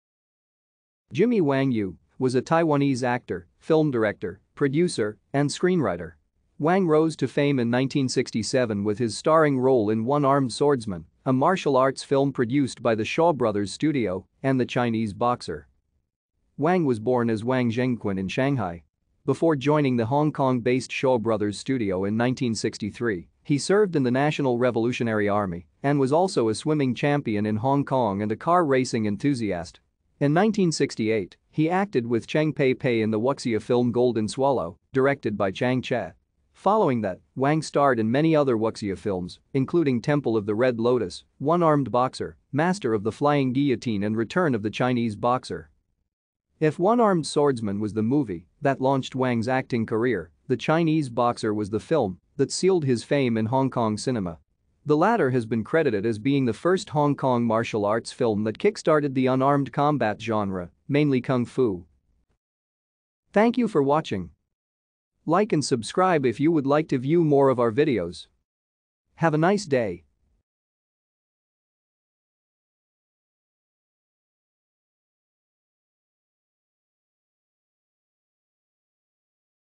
Jimmy Wang Yu
Jimmy Wang Yu, was a Taiwanese actor,  director, producer, and screenwriter. Wang rose to fame in 1967 with his starring role in One-Armed Swordsman, a martial arts  produced by the Shaw Brothers Studio, and The Chinese Boxer.